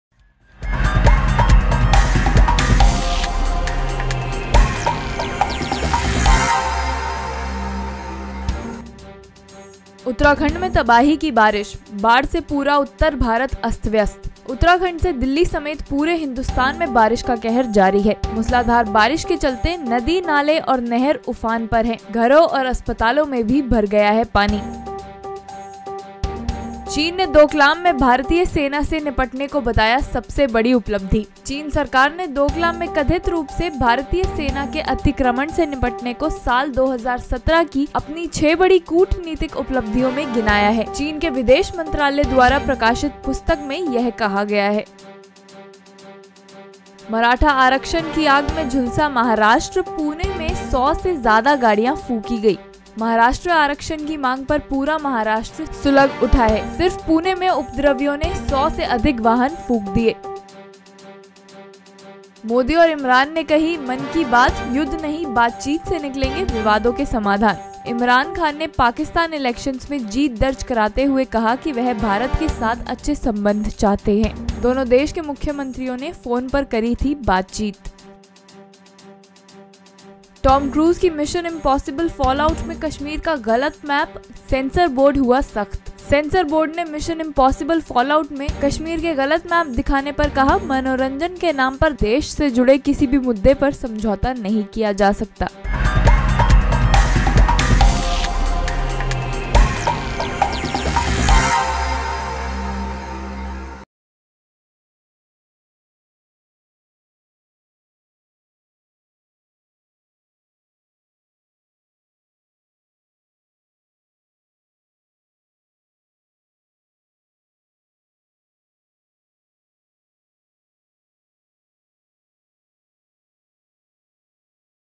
Top 5 News: देखिये आज की बड़ी खबरें | चीन की किताब में हुई भारत की प्रशंसा | Punjab Kesari
Watch this video for major news headlines.